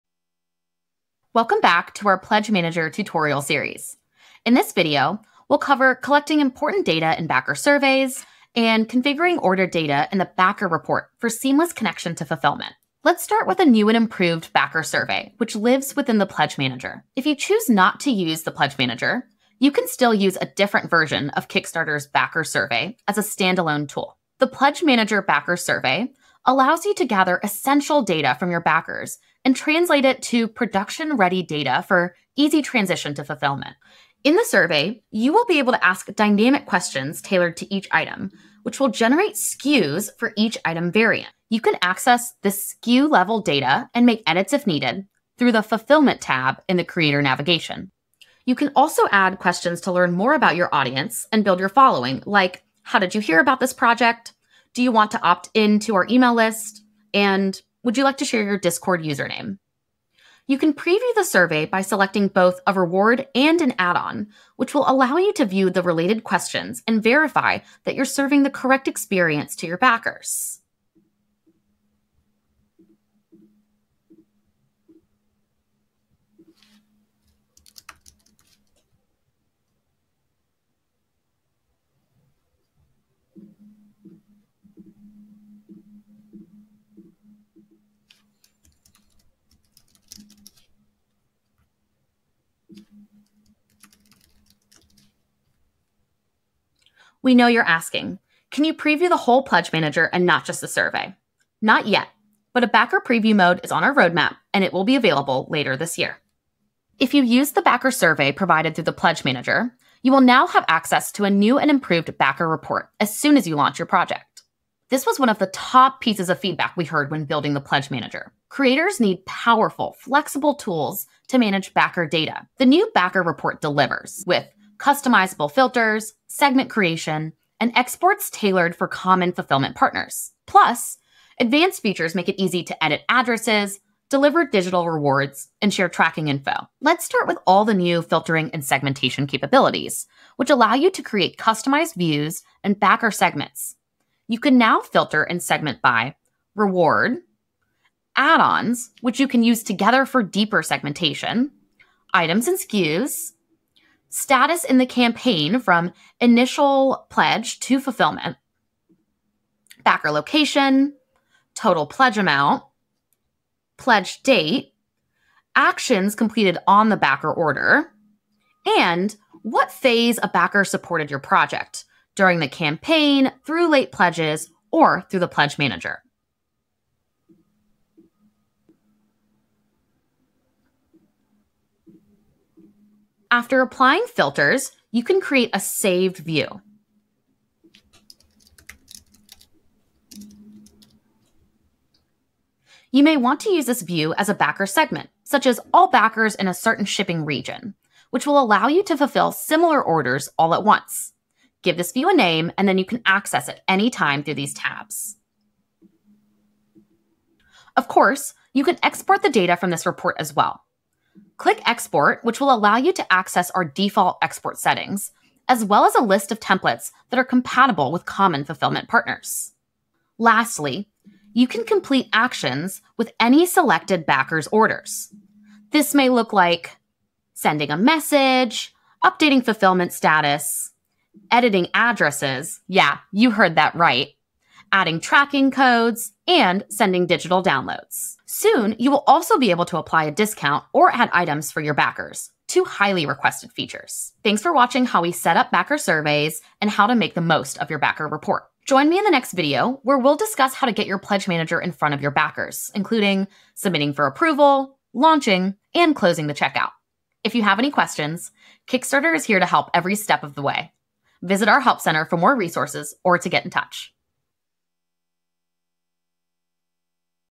Kickstarter Pledge Manager | Backer Survey and Backer Report Tutorial | Part 4
Introducing the new and improved Kickstarter Pledge Manager Backer Survey. In this video tutorial, we will cover how to collect important data in backer surveys and how to configure order data in your backer report to make the fulfillment process a breeze.

00:13: New Backer Survey
01:37: Backer Preview Mode Coming Soon
01:47: New Backer Report
02:17: Backer Report Filtering and Segmentation
03:03: Backer Report Saved View
03:28: Backer Report Exporting
03:40: Additional Backer Report Actions

Any Questions?

Kickstarter is here to make the world a more creative place. 💡